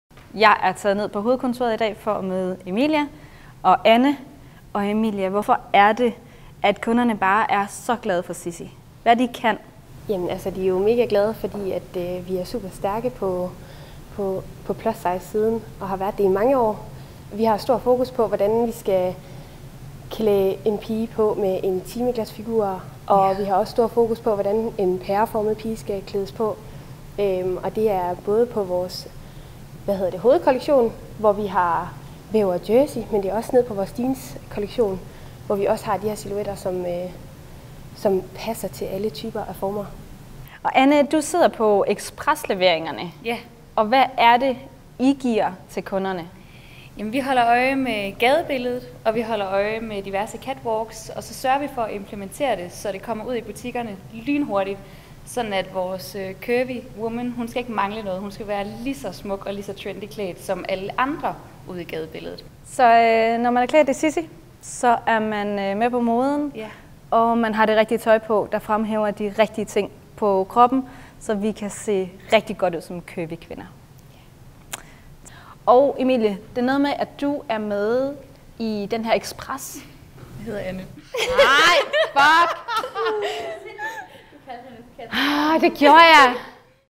zizzi hovedkontor
Jeg er taget ned på Zizzi's hovedkontor i dag for at møde Emilie og Anne.

Hvorfor er det at kunderne bare er så glade for Zizzi?

De er mega glade fordi Zizzi er super stærke på plus size, og har været det i mange år. Vi har stor fokus på hvordan vi skal klæde en pige på med en timeglas figur, og vi ved også godt hvordan en pæreformet pige skal klædes på. Det er både på Zizzi's hovedkollektion med væv og jersy, men også på jeans kollektion, med plads til alle typer og former.

Anne du sidder på Zizzi's express leveringer. Hvad er det I giver til kunderne?

Vi holder øje med galdebilledet og vi holder øje med diverse catwalks, og sørger for at implementere det, så det kommer ud i butikkerne lynhurtigt. En curvy woman skal ikke mangle noget. Hun skal være lige så trendy og ligeså godt klædt, som alle andre.

Når man er klædt i Zizzi, er man med på moden, og man har det rigtige tøj på, som fremhæver det rigtige på kroppen. Så vi kan se rigtig godt ud som curvy kvinder.